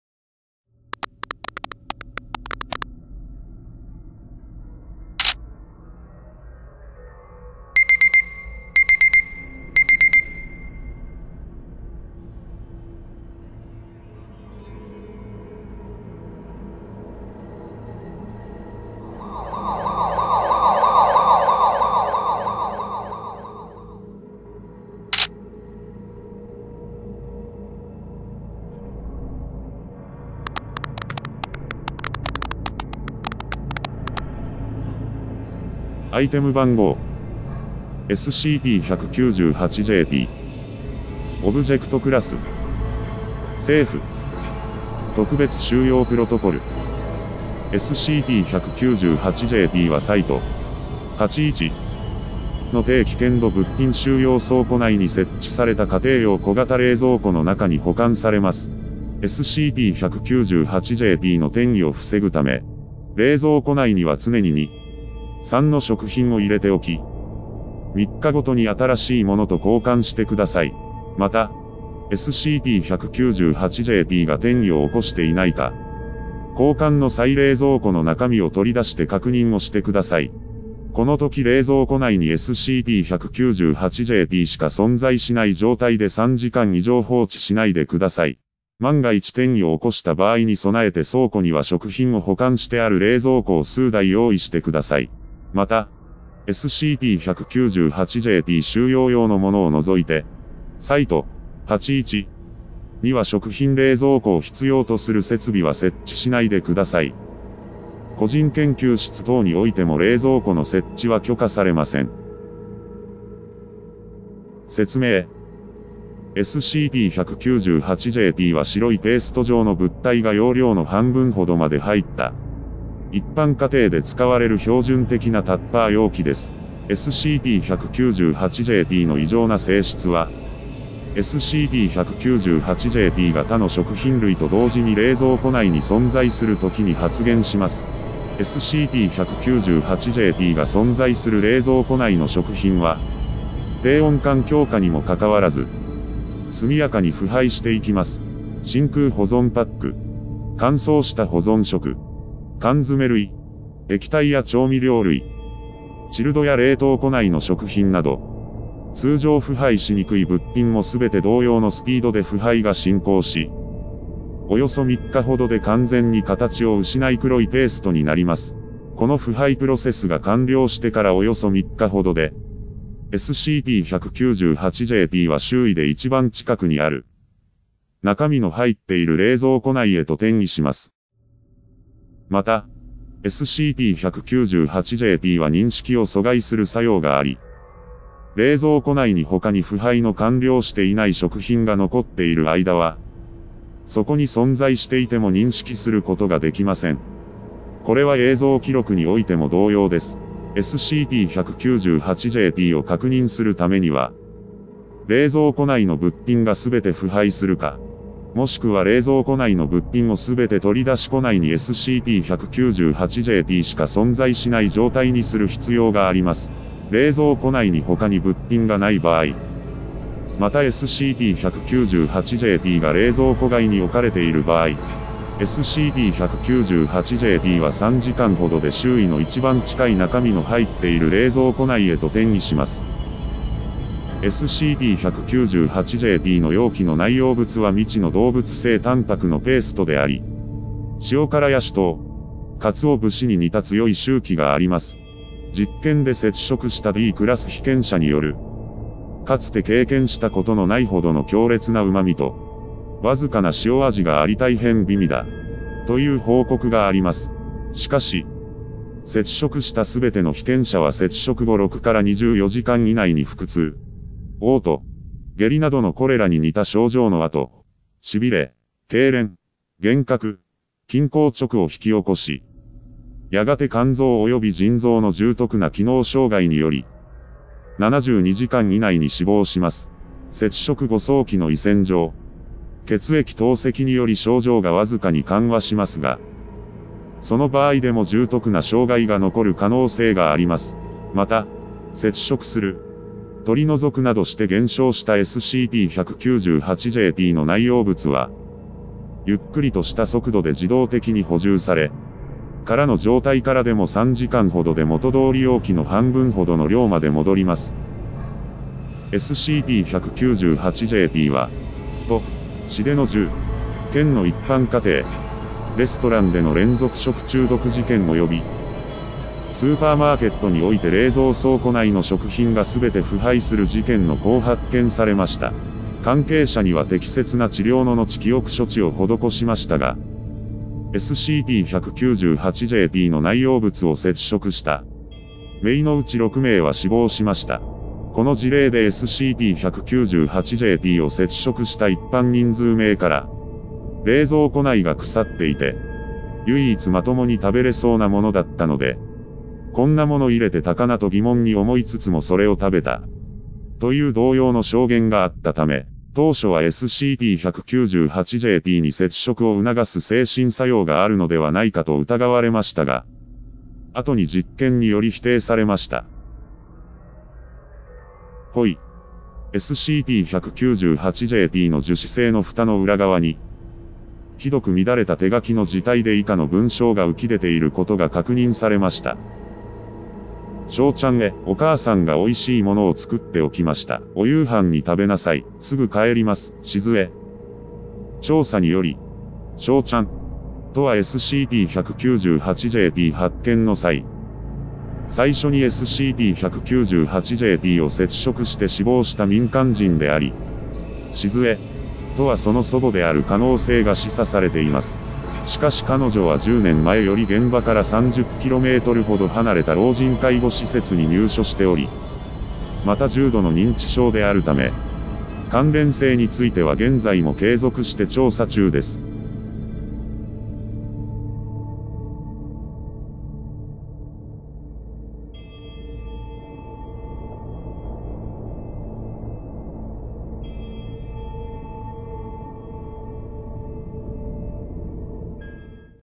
SCP財団機密データ：SCP-198-JP - 饐の素（すえのもと）
説明: SCP-198-JPは白いペースト状の物体が容量の半分ほどまで入った、一般家庭で使われる標準的なタッパー容器です。
SCP-198-JPの異常な性質は、SCP-198-JPが他の食品類と同時に冷蔵庫内に存在する時に発現します。SCP-198-JPが存在する冷蔵庫内の食品は、低温環境下にも関わらず、速やかに腐敗していきます。